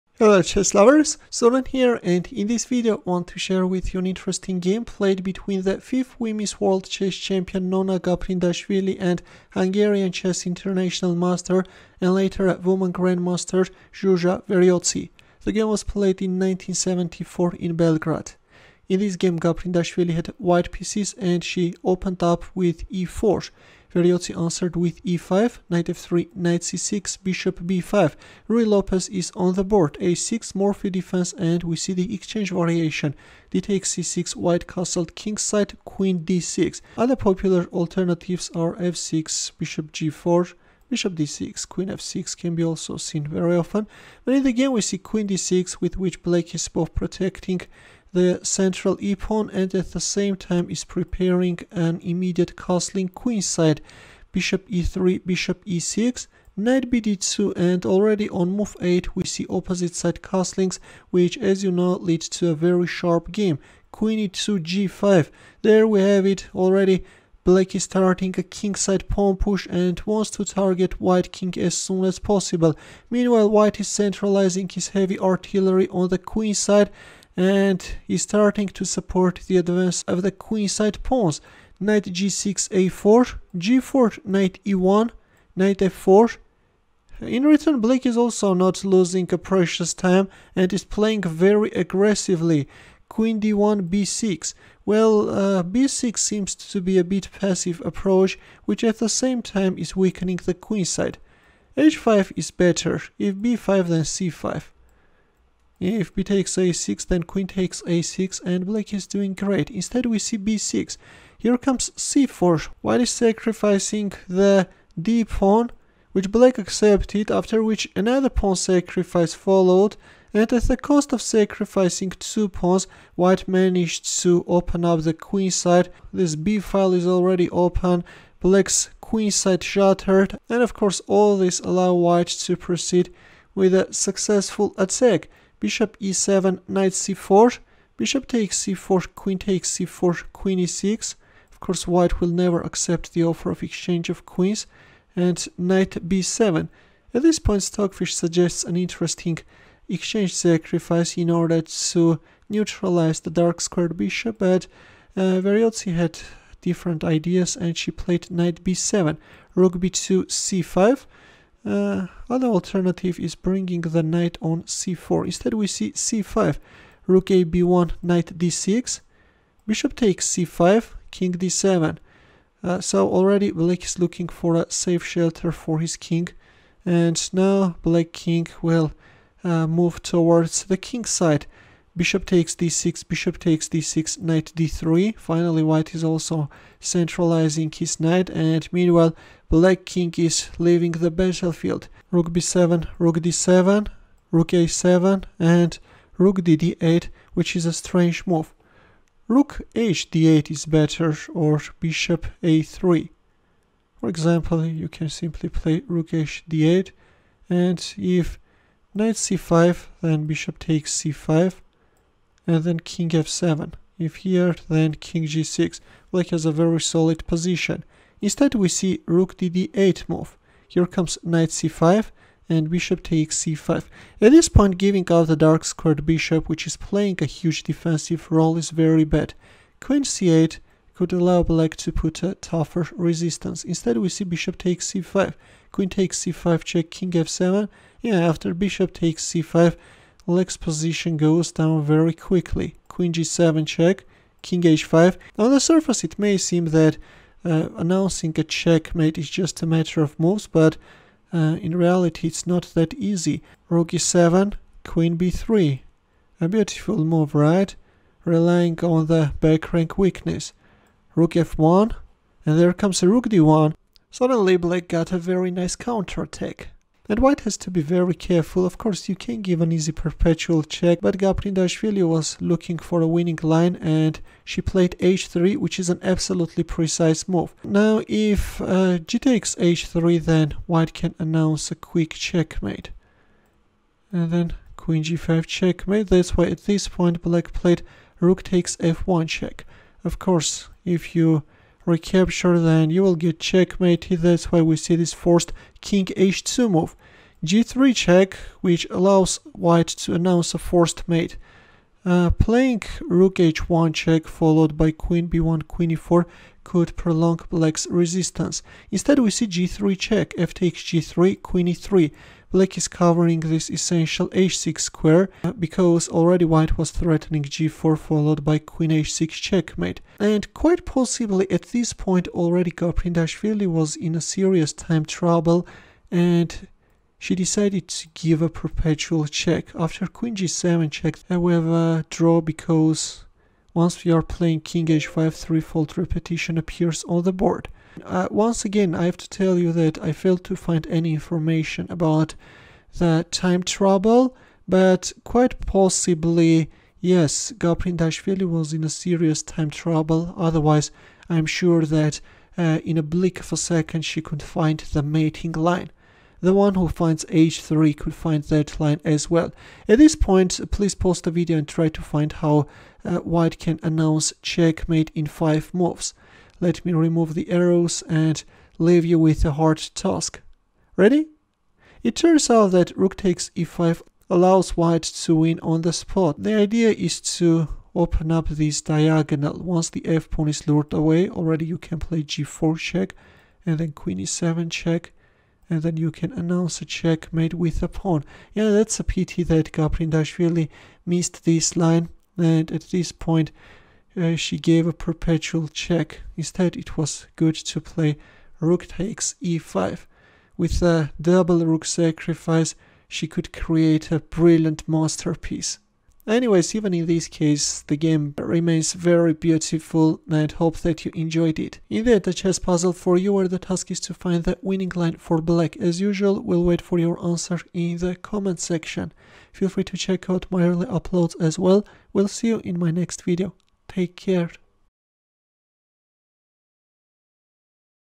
This Could Be Gaprindashvili's Masterpiece, But Alas...
The fifth women's world chess champion Nona Gaprindashvili sacrifices her rook, but misses the logical continuation which involves a second rook sacrifice!
Nona Gaprindashvili vs Zsuzsa Veroci Petronic
Jugoslavija (1974)
Spanish Game: Exchange. Bronstein Variation (C69)
1.e4 e5 2.Nf3 Nc6 3.Bb5 a6 4.Bxc6 dxc6 5.O-O Qd6 6.d3 f6 7.Be3
Be6 8.Nbd2 O-O-O 9.Qe2 g5 10.Rfb1 Ne7 11.b4 Ng6 12.a4 g4
13.Ne1 Nf4 14.Qd1 b6 15.c4 Nxd3 16.c5 bxc5 17.bxc5 Nxc5 18.Qc2
Be7 19.Nc4 Bxc4 20.Qxc4 Qe6 21.Qc2 Nb7 22.Rb2 c5 23.Rab1 Nd6
24.Bxc5 Kd7 25.Bxd6 Bxd6 26.Nd3 Ke7 27.Rb7 Rd7 28.Ra7 Rdd8
29.Nc5 Bxc5 30.Qxc5+ Kf7 31.Qxc7+ Kg6 32.Qg7+ Kh5 33.Re7 Qb3
34.Rf1 Rd1 35.h3 Rxf1+ 36.Kh2 g3 37.fxg3 Qe3 38.Qg4+ Kh6
39.Qg7+ Kh5 40.Qg4+ Kh6 41.Qg7+
1/2-1/2
Introduction: 0:00
Critical Position 1: 4:26
Critical Position 2: 5:59
Daily Puzzle: 6:51